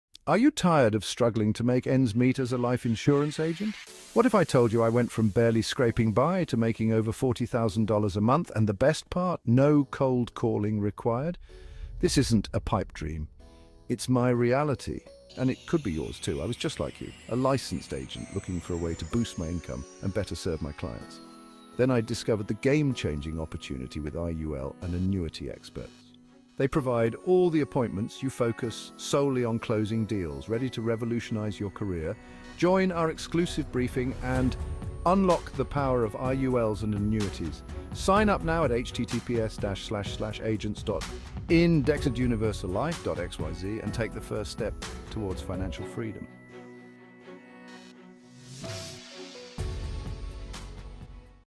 How I Earn $40k+ Monthly Without Cold Calling
Are you ready to revolutionize your career as a life insurance agent? Discover how to turn your reality into $40,000+ months without cold calling. Join us for an exclusive briefing and unlock the power of Indexed Universal Life (IUL) policies and annuities. Learn how to secure your clients' financial futures while dramatically increasing your income.